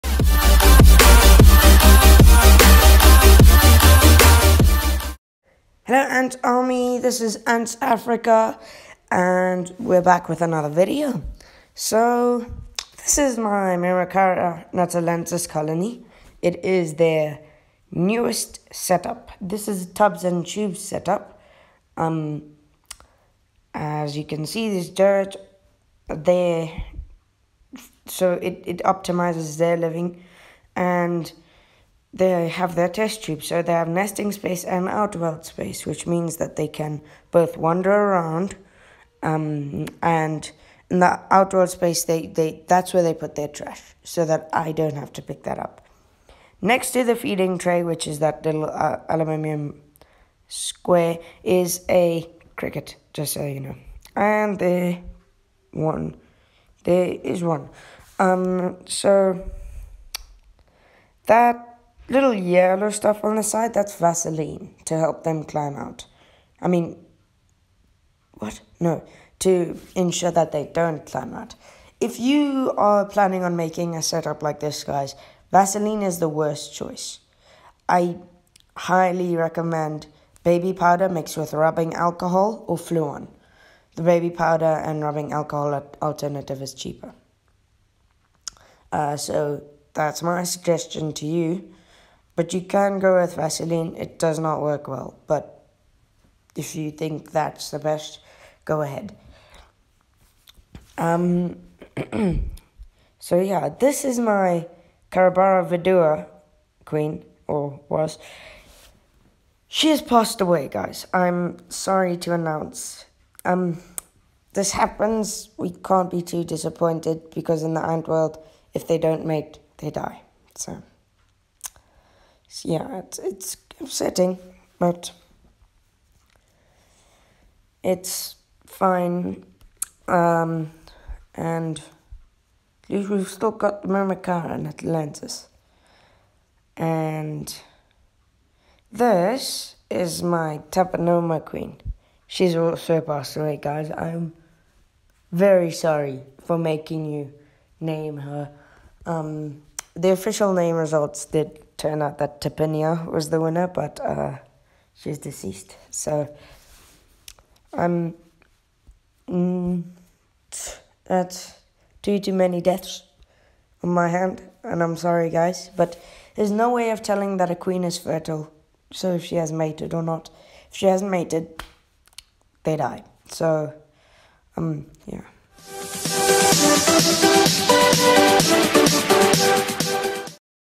My Myrmicaria colony’s new setup!
Hi Ant Army! In today’s video we’ve got good news, sad news and all in between! I hope you enjoyed!